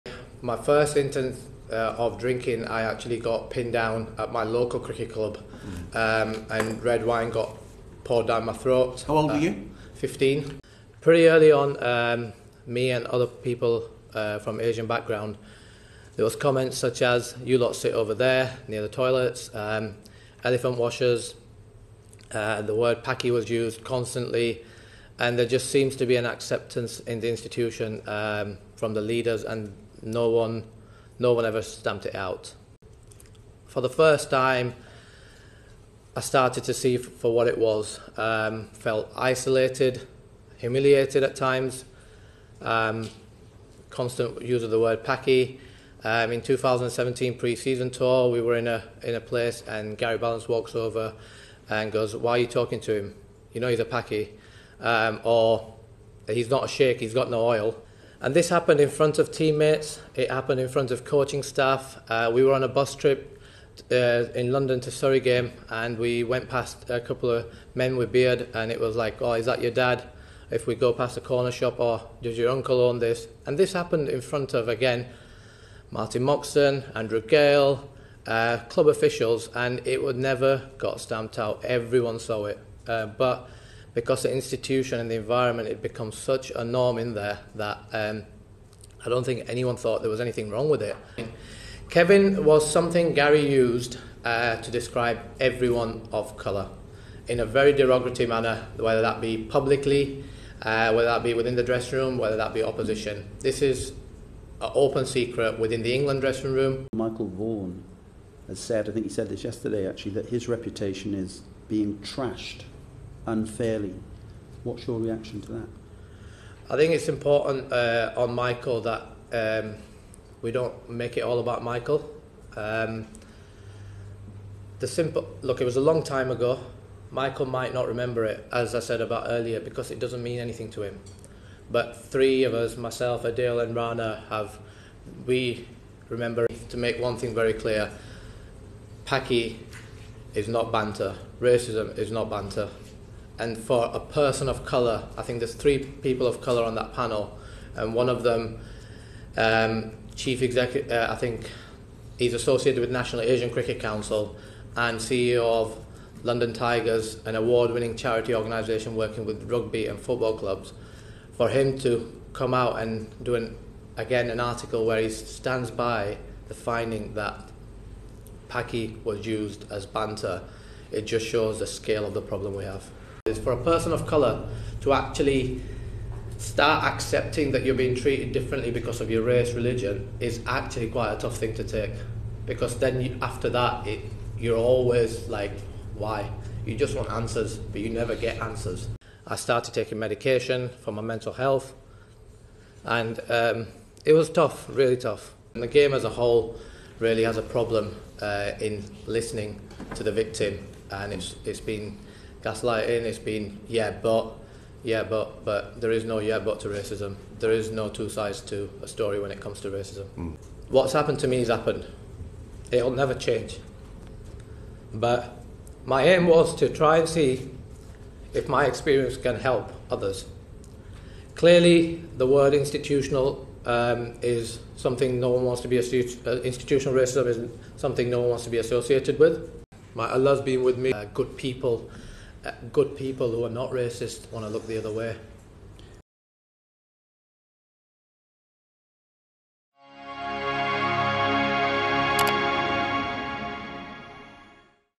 Racism in Cricket - Select Committee Hearing with Azeem Rafiq
Highlights - Digital, Culture, Media and Sport Committee